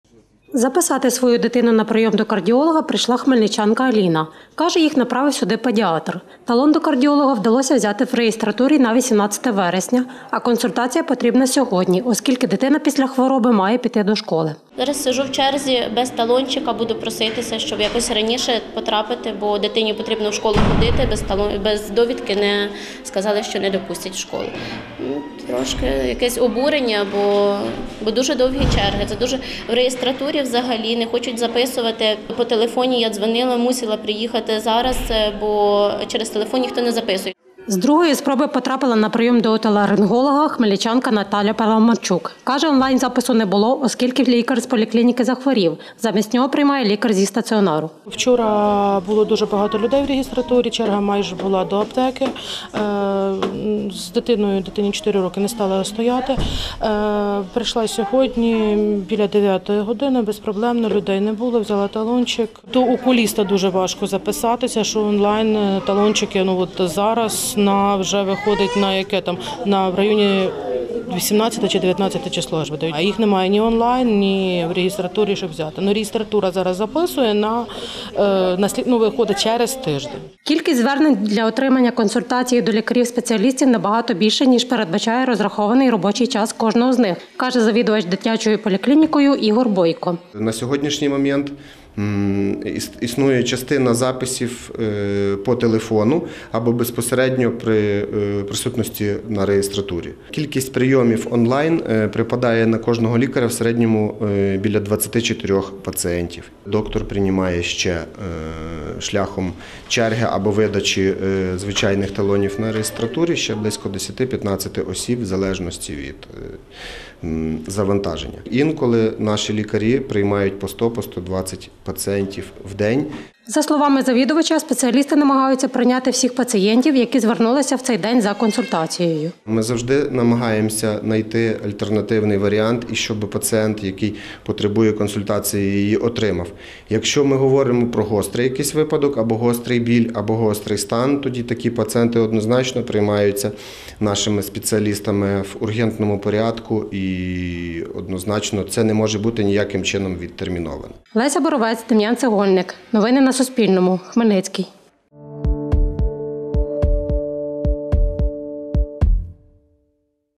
Хмельничани чекають два тижні, аби потрапити з дитиною на прийом до лікаря-спеціаліста
Записати дитину на прийом до вузького спеціаліста у поліклініку онлайн чи телефоном неможливо, а талони розібрали за один-два тижні наперед. Люди годинами стояти в живій черзі, щоб потрапити на прийом до лікаря. Така ситуація склалася у Хмельницькій міській дитячій поліклініці, розповідають хмельничани у соцмережах.